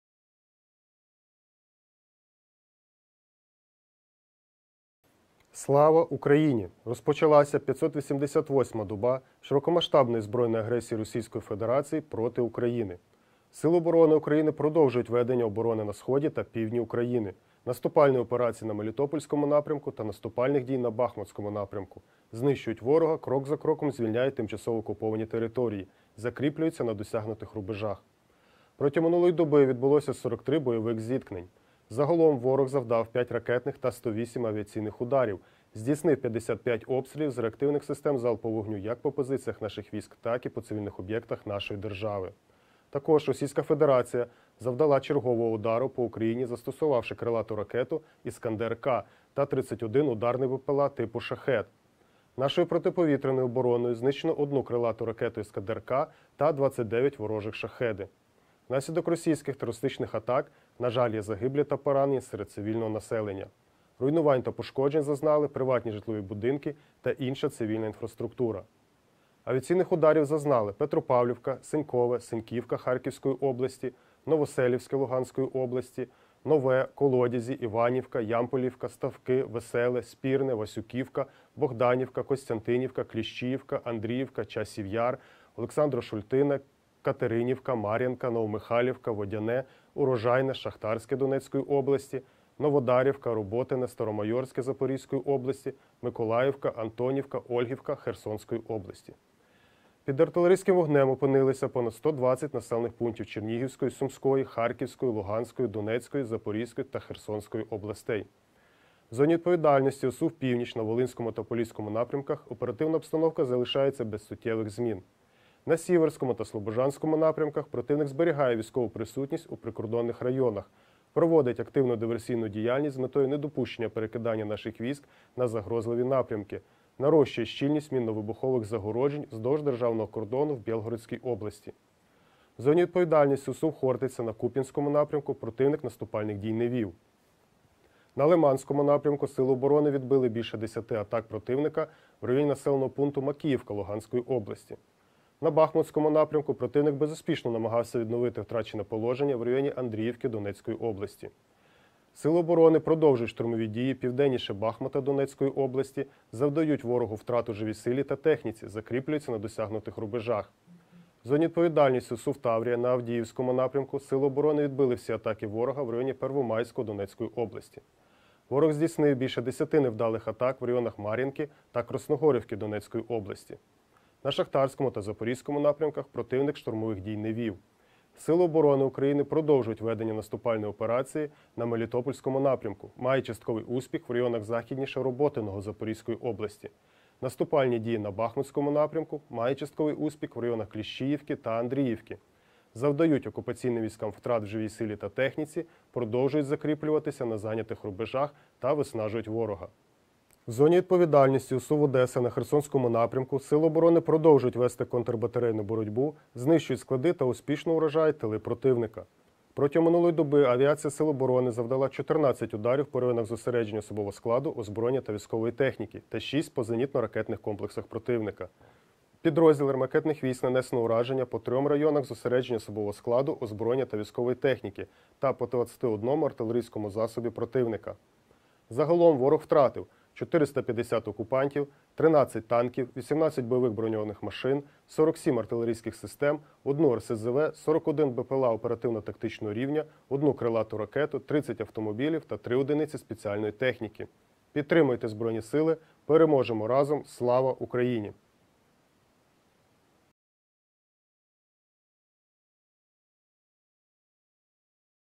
Оперативна інформація станом на 10.00 04 жовтня 2023 року щодо російського вторгнення
Речник Генерального штабу Збройних Сил України Павло Ковальчук

Слава Україні! Розпочалася п’ятсот вісімдесят восьма доба широкомасштабної збройної агресії російської федерації проти України.

Сили оборони України продовжують ведення оборони на сході та півдні України, наступальної операції на Мелітопольському напрямку та наступальних дій на Бахм  утському напрямку, знищують ворога, крок за кроком звільняють тимчасово окуповані території, закріплюються на досягнутих рубежах.

Протягом минулої доби відбулося 43 бойових зіткнень. Загалом, ворог завдав 5 ракетних та 108 авіаційних ударів, здійснив 55 обстрілів з реактивних систем залпового вогню як по позиціях наших військ, так і по цивільних об’єктах нашої держави. Також, російська федерація завдала чергового удару по Україні застосувавши крилату ракету «Іскандер-К» та 31 ударний БпЛА типу “Shahed-136/131”. Нашою протиповітряною обороною знищено 1 крилату ракету «Іскандер-К» та 29 ворожих “шахеди”. Внаслідок російських терористичних атак, на жаль, є загиблі та поранені серед цивільного населення. Руйнувань та пошкоджень зазнали приватні житлові будинки та інша цивільна інфраструктура.

Авіаційних ударів зазнали: Петропавлівка, Сенькове, Синьківка Харківської області; Новоселівське Луганської області, Нове, Колодязі, Іванівка, Ямполівка, Ставки, Веселе, Спірне, Васюківка, Богданівка, Костянтинівка, Кліщіївка, Андріївка, Часів Яр, Олександро-Шультине, Катеринівка, Мар’їнка, Новомихайлівка, Водяне, Урожайне, Шахтарське Донецької області; Новодарівка, Роботине, Старомайорське Запорізької області; Миколаївка, Антонівка, Ольгівка Херсонської області.

Під артилерійським вогнем опинилися понад 120 населених пунктів Чернігівської, Сумської, Харківської, Луганської, Донецької, Запорізької та Херсонської областей.

В зоні відповідальності ОСУВ “Північ” на Волинському та Поліському напрямках оперативна обстановка залишається без суттєвих змін.

На Сіверському та Слобожанському напрямках противник зберігає військову присутність у прикордонних районах, проводить активну диверсійну діяльність з метою недопущення перекидання наших військ на загрозливі напрямки, нарощує щільність мінно-вибухових загороджень  вздовж державного кордону в бєлгородській області.

В зоні відповідальності ОСУВ “Хортиця” на Куп’янському напрямку противник наступальних (штурмових) дій не вів.

На Лиманському напрямку сили оборони відбили більше 10 атак противника в районі населеного пункту Макіївка Луганської області.

На Бахмутському  напрямку противник безуспішно намагався відновити втрачене положення в районі Андріївки Донецької області. Сили оборони продовжують штурмові дії південніше Бахмута Донецької області, завдають ворогу втрат у живій силі та техніці, закріплюються на досягнутих рубежах.

В зоні відповідальності ОСУВ “Таврія” на Авдіївському напрямку Сили оборони відбили всі атаки ворога в районі Первомайського Донецької області. Ворог здійснив більше 10 невдалих атак в районах Мар’їнки та Красногорівки Донецької області.

На Шахтарському та Запорізькому напрямках противник наступальних (штурмових) дій не вів.

Сили оборони України продовжують ведення наступальної операції на Мелітопольському напрямку, мають частковий успіх в районах західніше Роботиного Запорізької області, наступальні (штурмові) дії на Бахмутському напрямку, мають частковий успіх в районах Кліщіївки та Андріївки, завдають окупаційним військам втрат в живій силі та техніці, продовжують закріплятися на зайнятих рубежах та виснажують ворога.

В зоні відповідальності ОСУВ “Одеса” на Херсонському напрямку Сили оборони продовжують вести контрбатарейну боротьбу, знищують склади та успішно уражають тили противника.

Протягом минулої доби авіація сил оборони завдала 14 ударів по районах зосередження особового складу, озброєння та військової техніки та 6  по зенітно-ракетних комплексах противника.

Підрозділами ракетних військ нанесено ураження по 3 районах зосередження особового складу, озброєння та військової техніки та по 21 артилерійському засобі противника.

Загалом, ворог втратив: 450 окупантів, 13 танків, 18 бойових броньованих машин, 47 артилерійських систем, одну РСЗВ, 41 БПЛА оперативно-тактичного рівня, одну крилата ракета, 30 автомобілів та 3 одиниці спеціальної техніки.

Підтримуйте Збройні Сили! Переможемо разом!
Слава Україні!